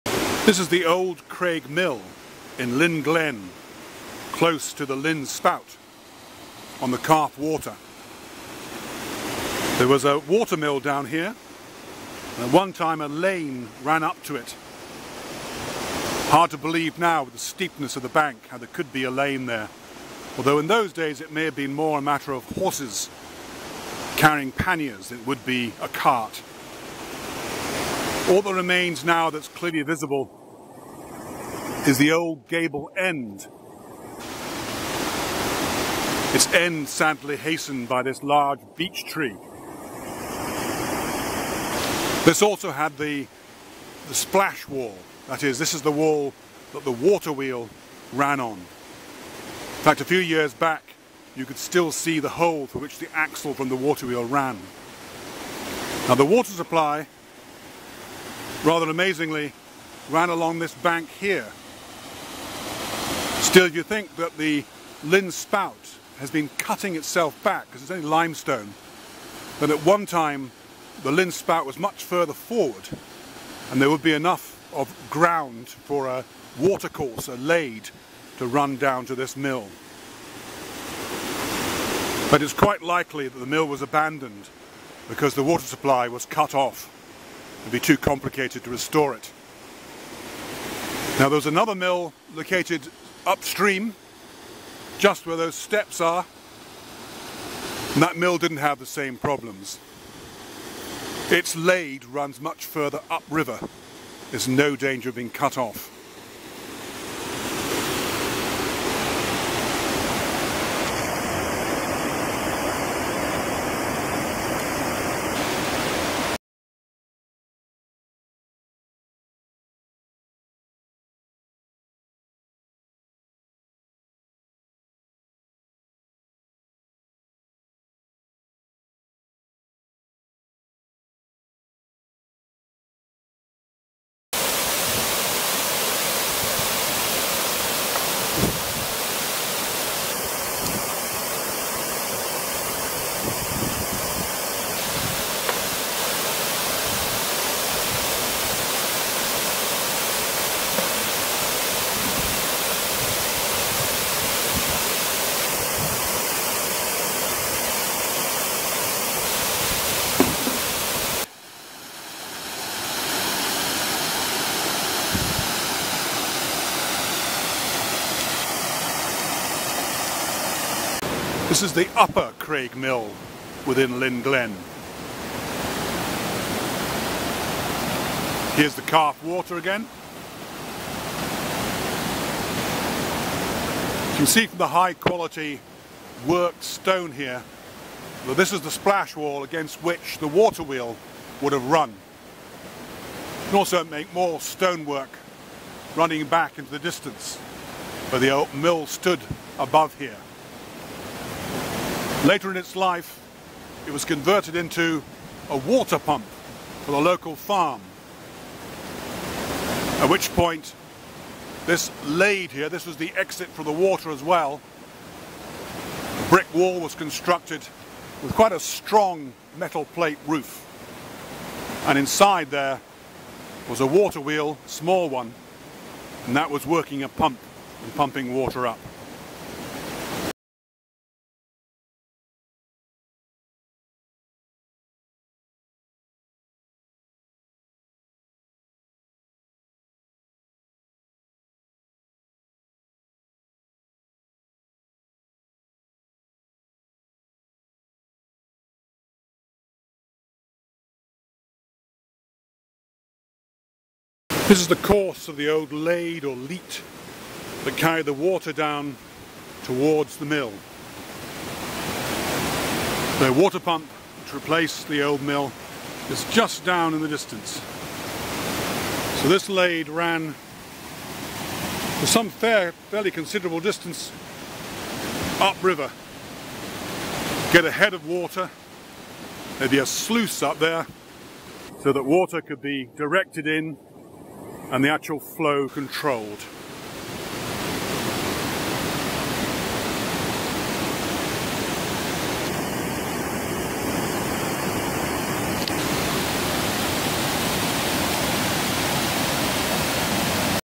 Water Mills on the Caaf Water, Lynn Glen, Dalry - History Insights
The Lyn Glen near Dalry had two water mills powered by the Caaf Water. One took its water from the Lynn Spout and the other via a lade. The monks of Kilwinning Abbey operated at least one of these mills. The mill on the lade was converted to a water pump.